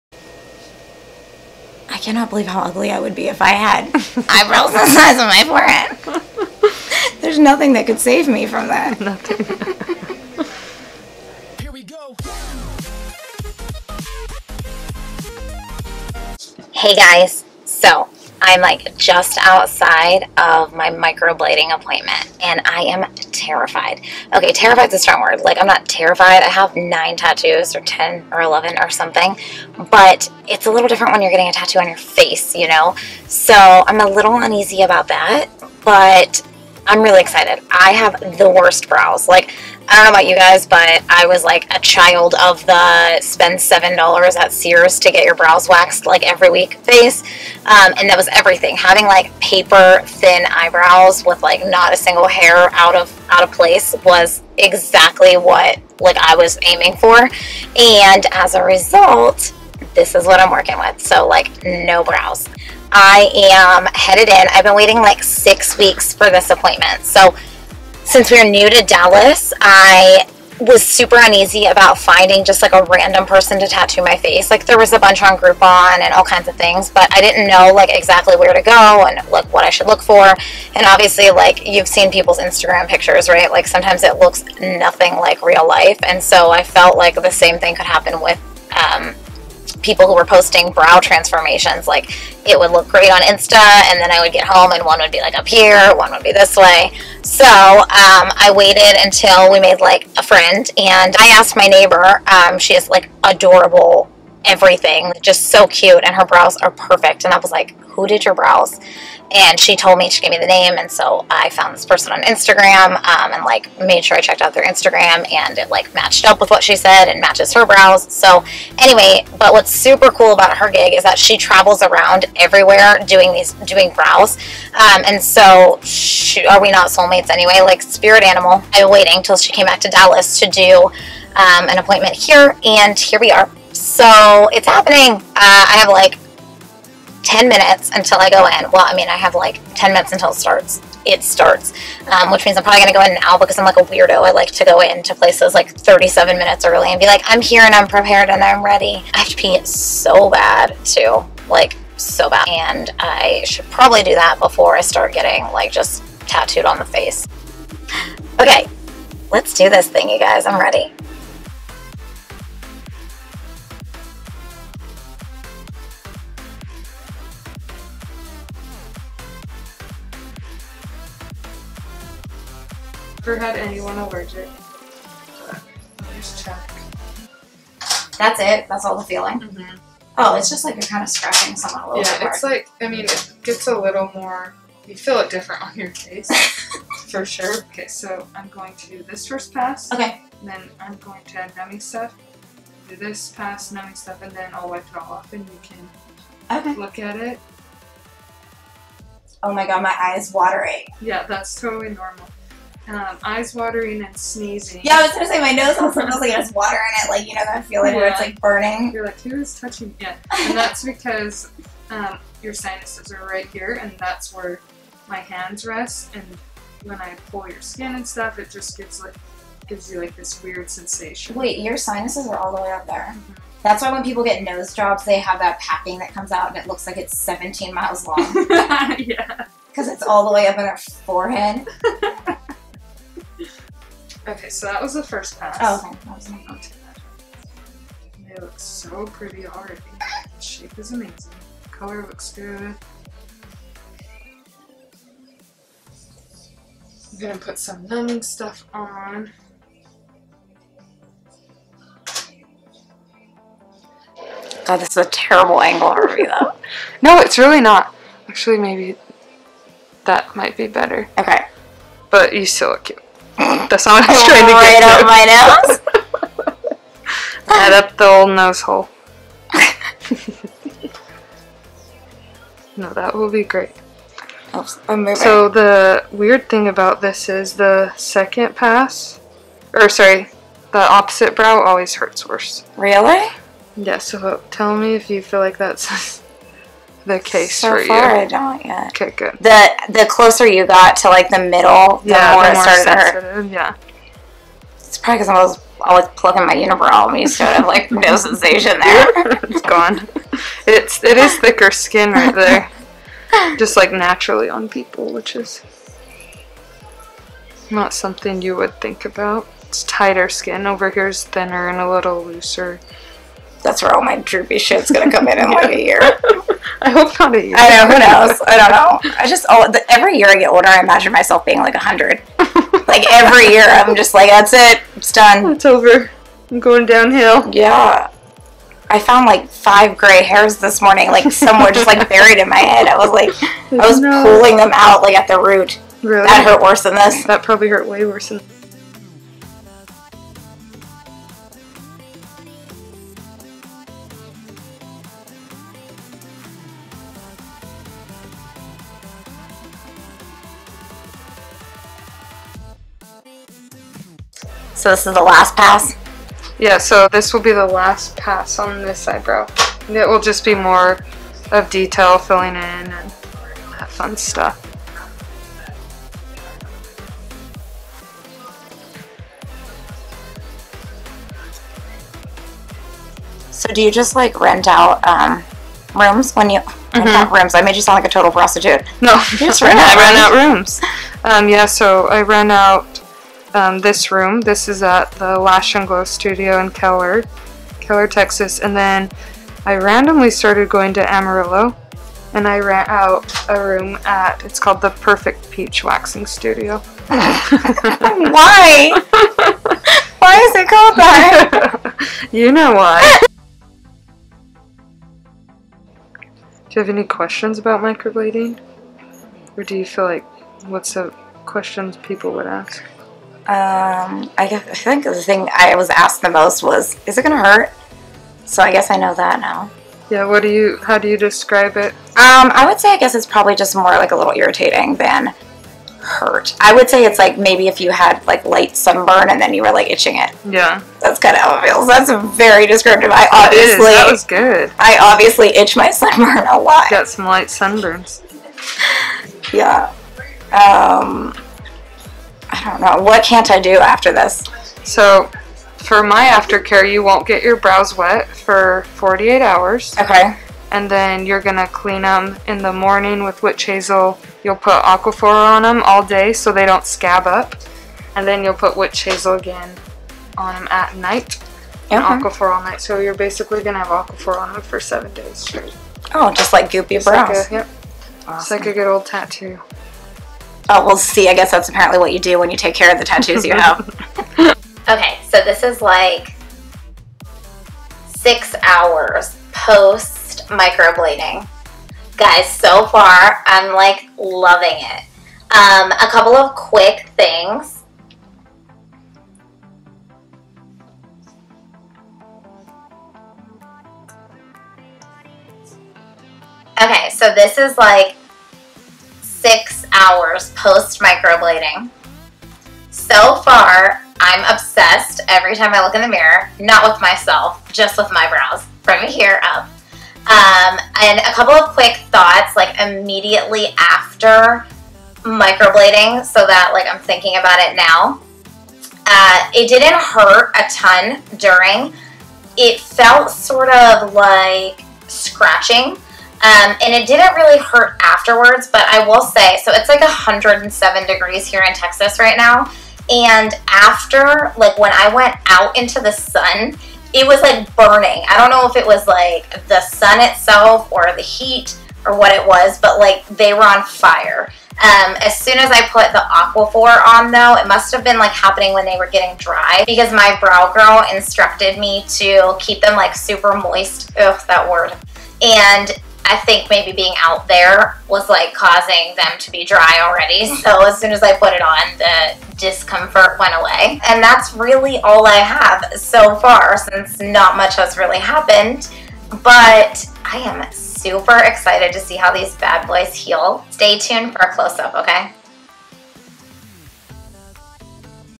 A Behind the Scenes Look at Microblading
This is such a fun one for me to share, because I had the BEST microblading experience, and I’m so glad that I finally took the leap and did this.

Honestly, I had no idea what to expect going into it, and I wanted to capture an in-depth look at this process because sharing is caring! This shows an up close and personal look at microblading from start to finish.

Be sure to tag along on Instagram to see how they look throughout the healing process!

•Resources—
Microblading Artist: Amy Baker

•Intro—
Background Pink Marble created by Freepik
Font Argentinian Nights by Kestrel Montes
Font Bellamy Doodles